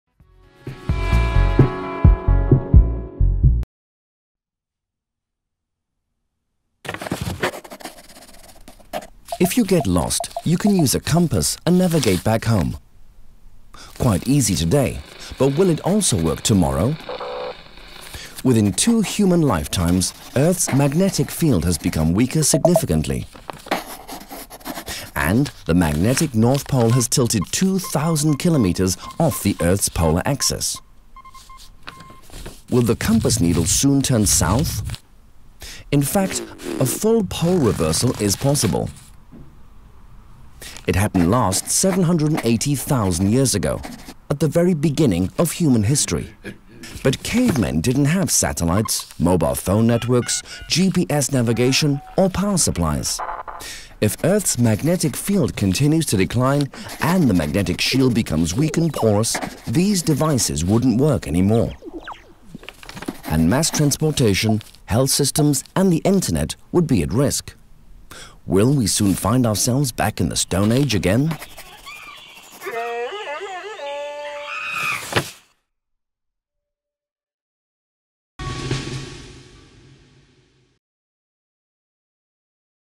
Magnetic field: why it matters
Earth's magnetic field is continuously changing. Magnetic north wanders, and every few hundred thousand years the polarity gradually flips, so that a compass would point south instead of north.